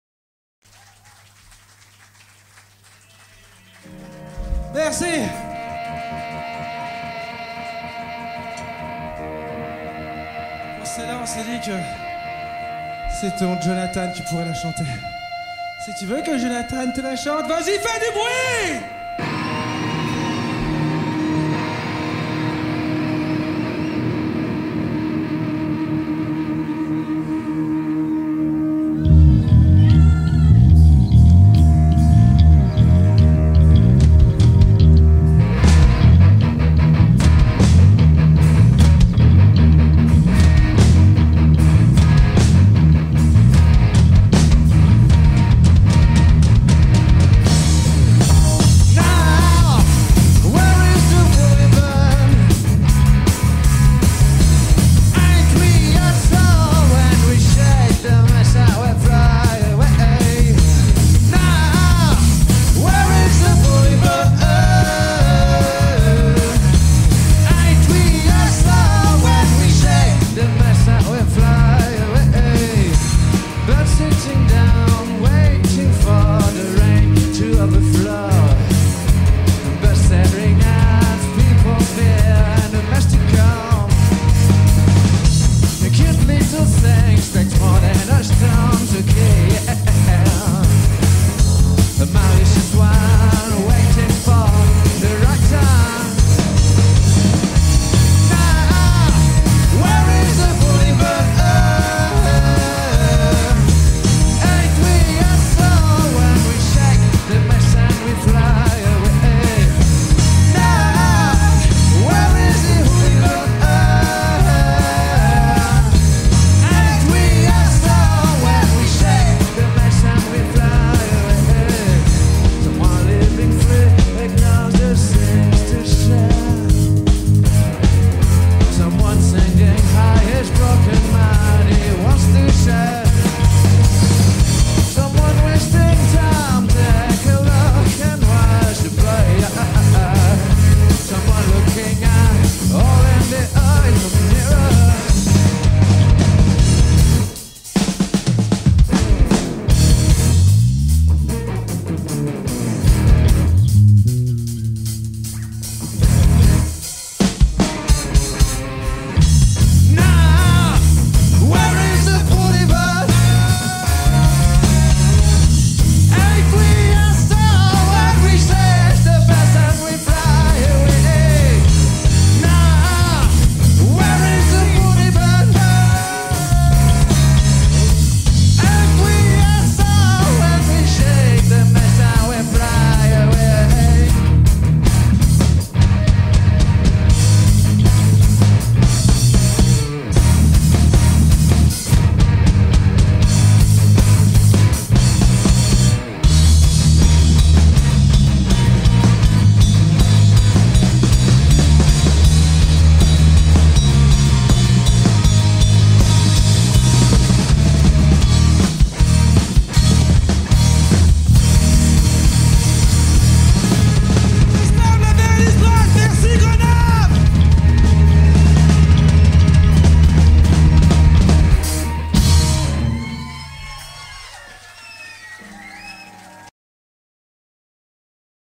Bully Bird @ l' Amperage 2019
Un extrait de notre passage à l'Ampérage, le désormais traditionnel morceau final avec de la sueur, des saut et un Jo bouillant au chant.